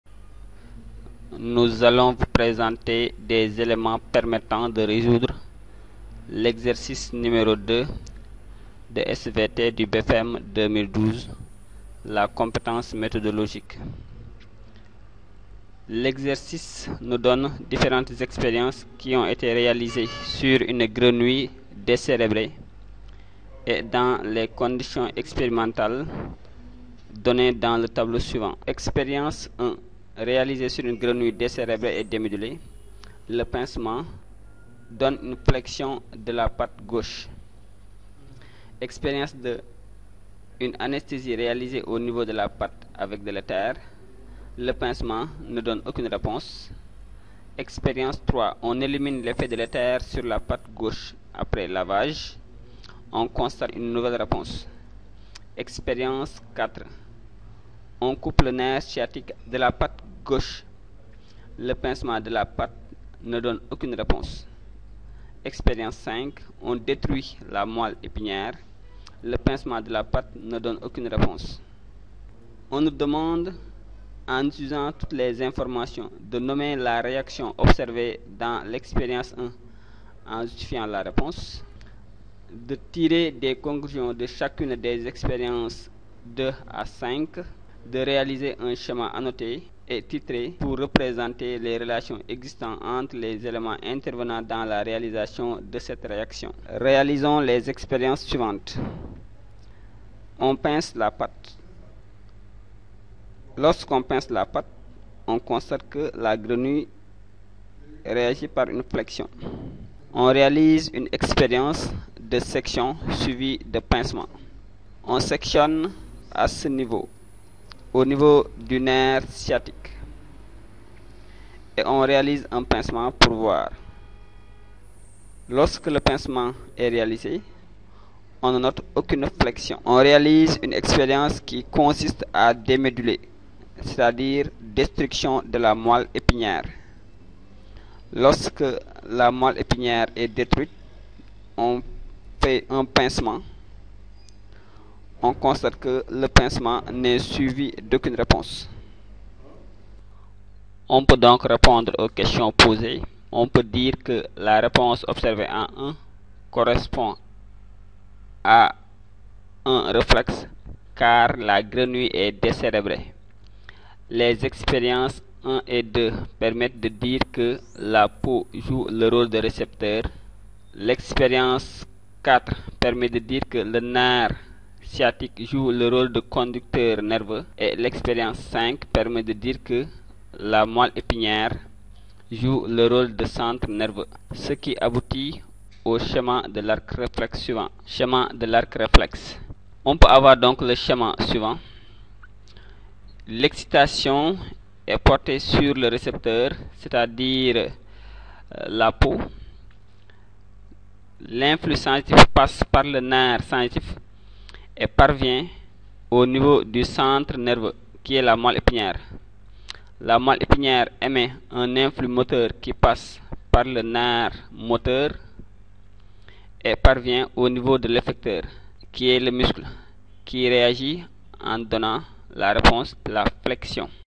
exo2 BFEM 2012
ressource qui aide a résoudre l'exercice portant sur le reflexe de flexion. elle est accompagnée  d'animations permettant de visualiser les éléments intervenant dans un reflexe